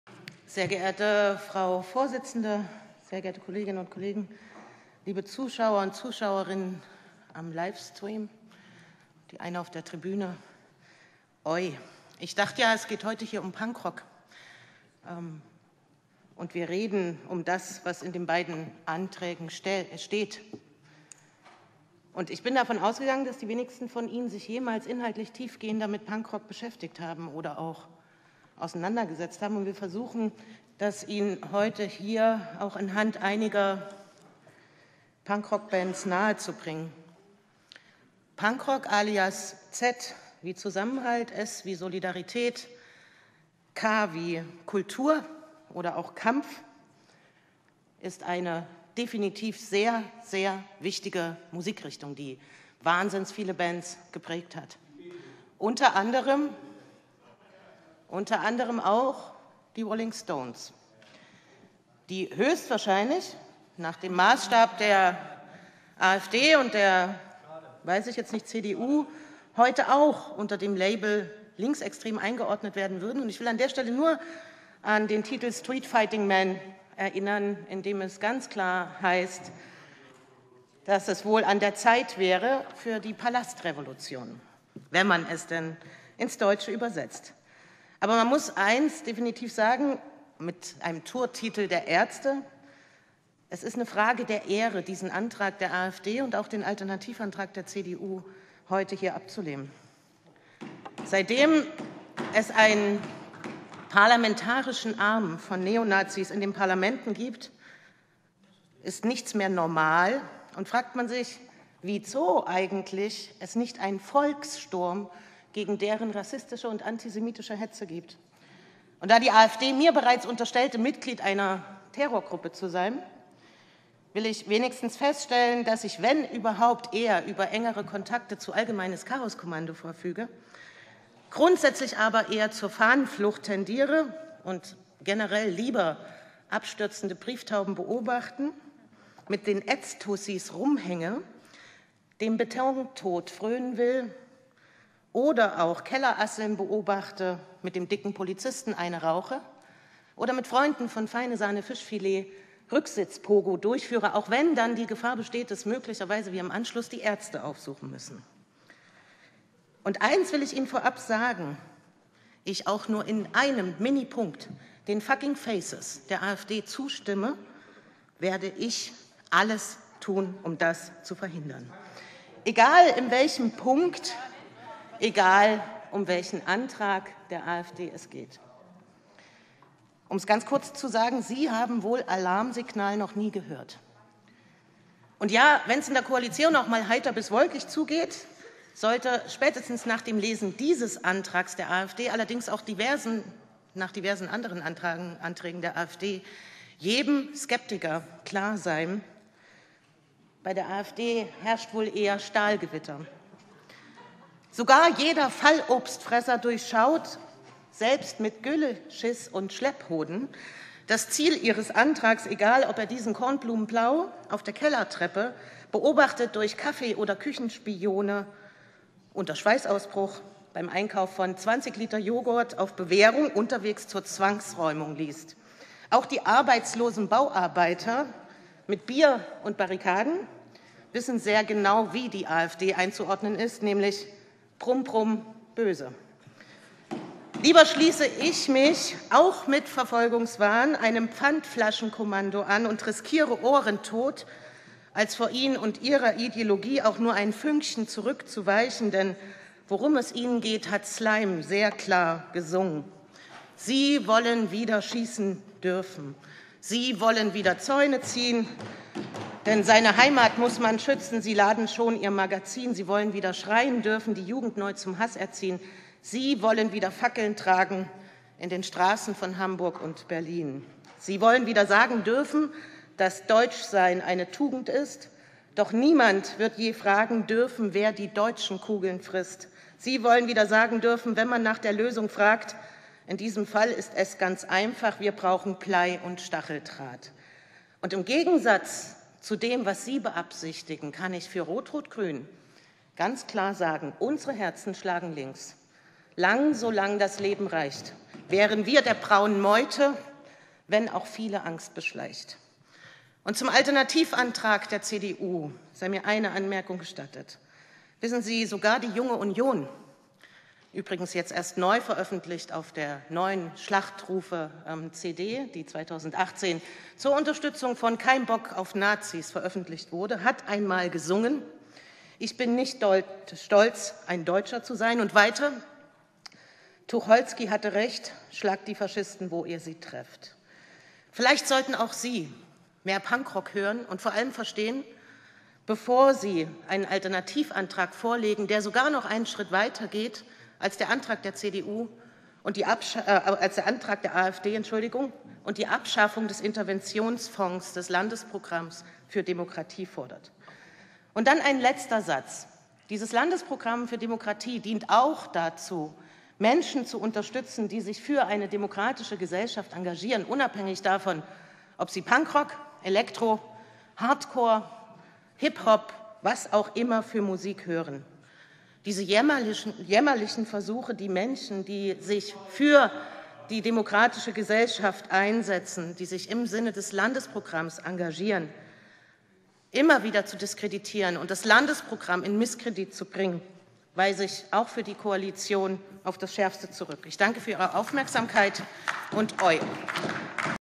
Punkrock alias Z wie Zusammenhalt S wie Solidarität und K wie Kultur oder auch Kampf
Im vorliegenden Antrag der AfD am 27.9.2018 im Thüringer Landtag ging es um Punkrock. MdL Katharina König versuchte den Abgeordneten einige Punkrockbands näher zu bringen.